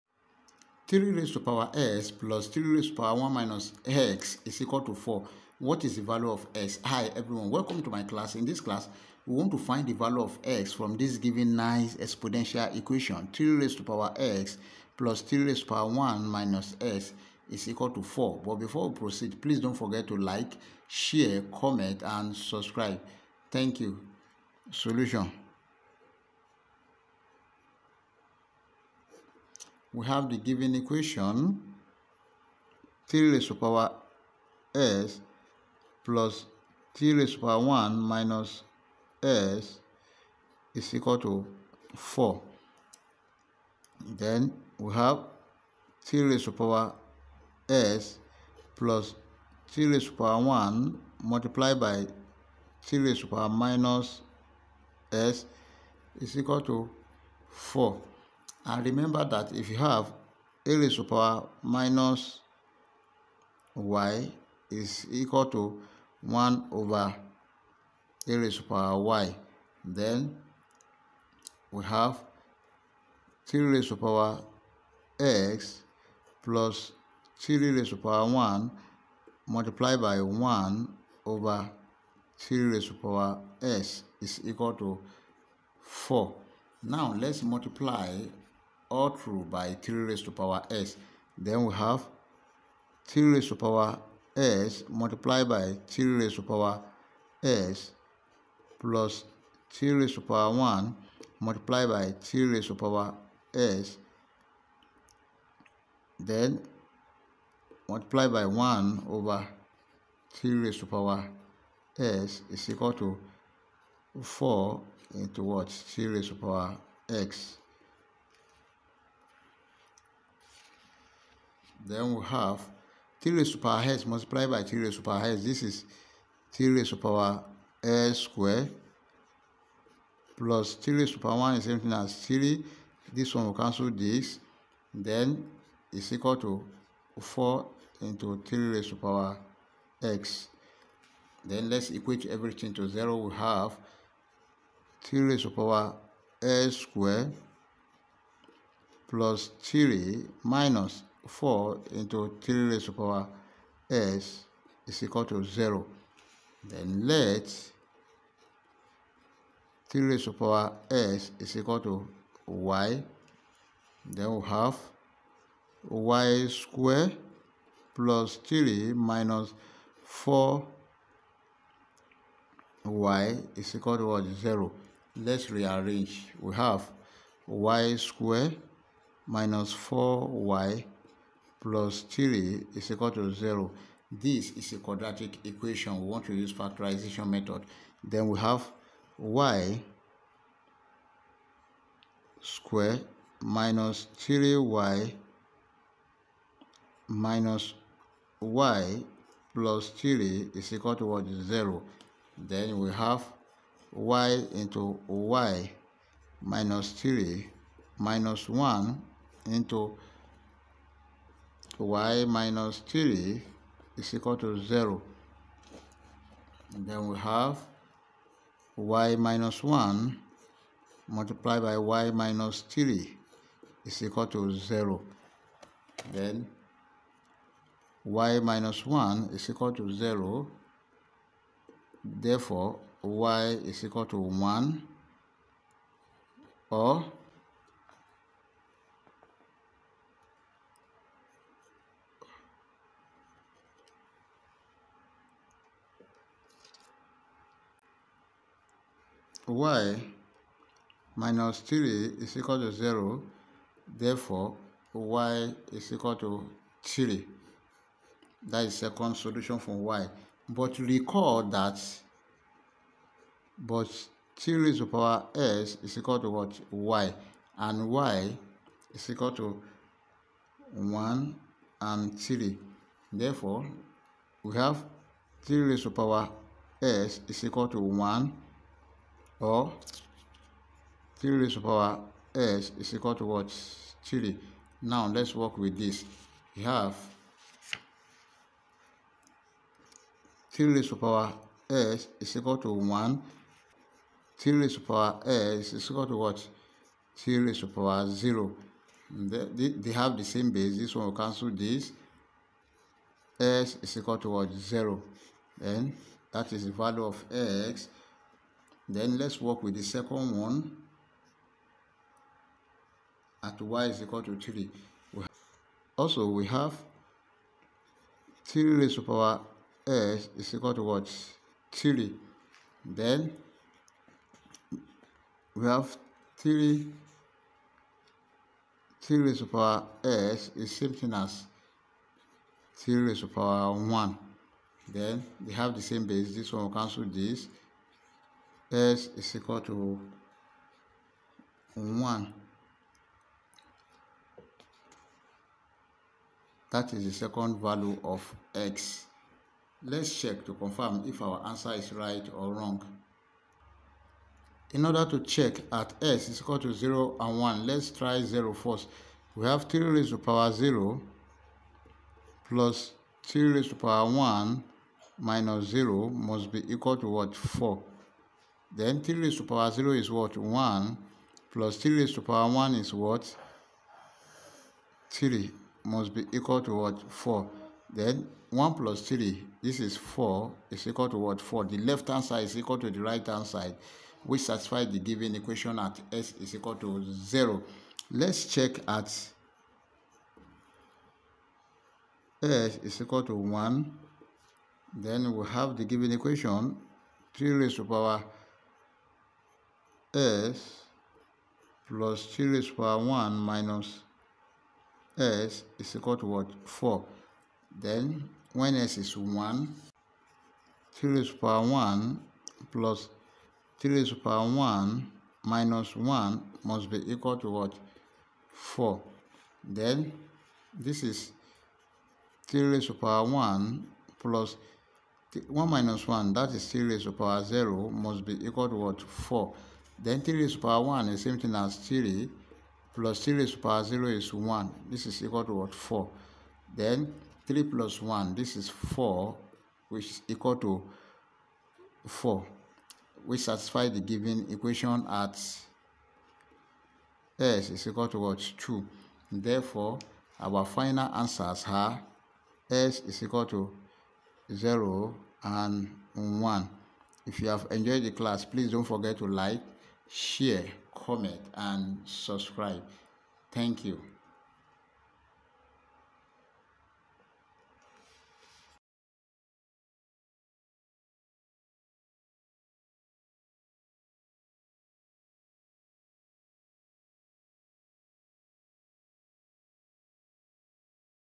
A Nice Exponential Equation 3^x+3^1-x=4
In This video, you will learn how to solve nice exponential equation very fast with this best trick.